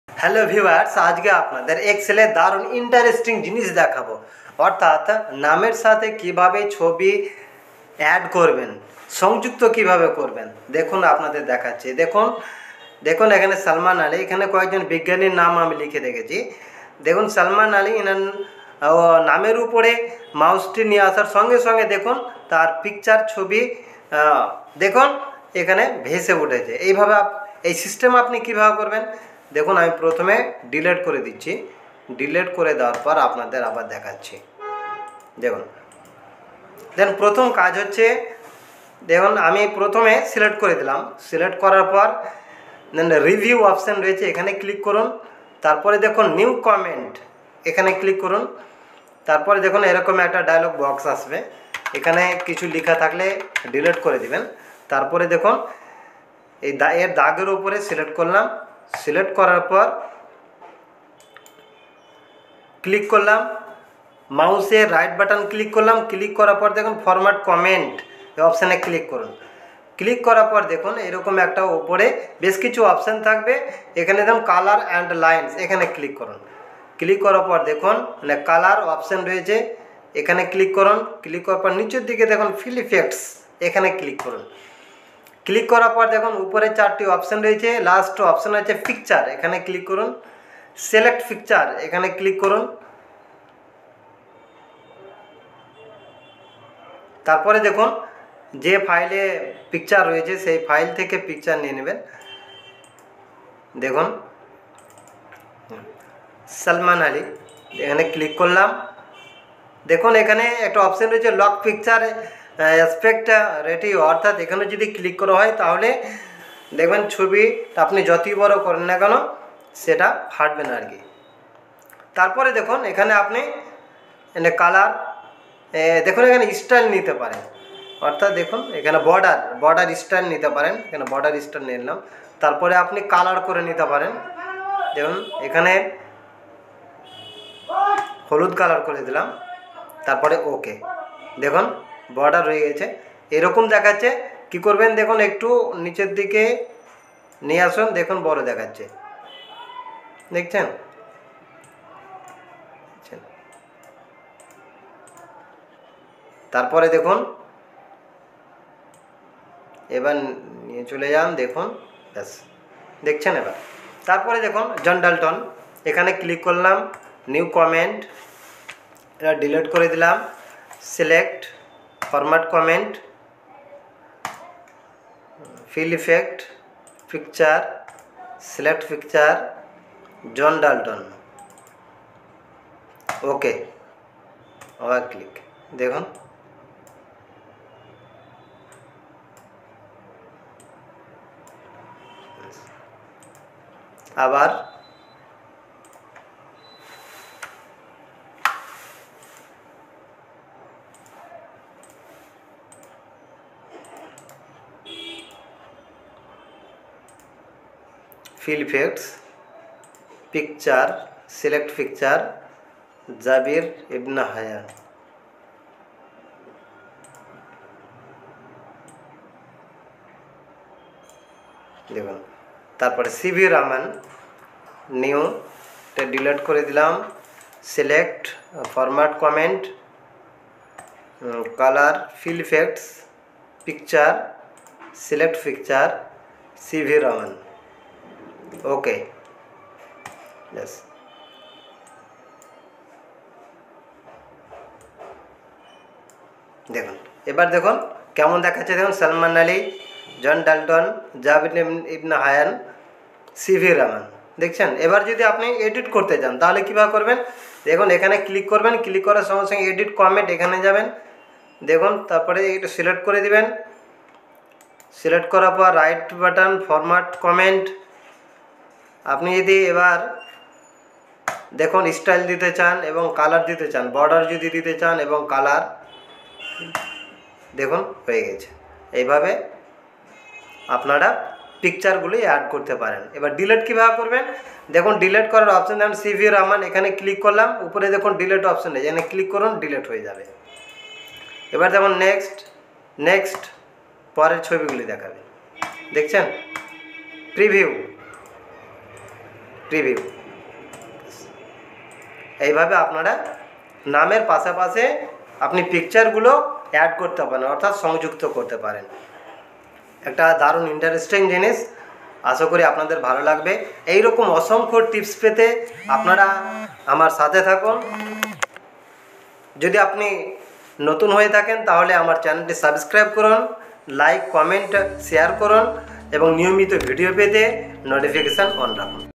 How to Insert Picture with name List in Excel | নামের সাথে এক্সেলে ছবি সংযুক্ত করুন
How to Insert a Picture on Name List in Excel | নামের সাথে এক্সেলে ছবি সংযুক্ত করুন
how to add picture with name list in excel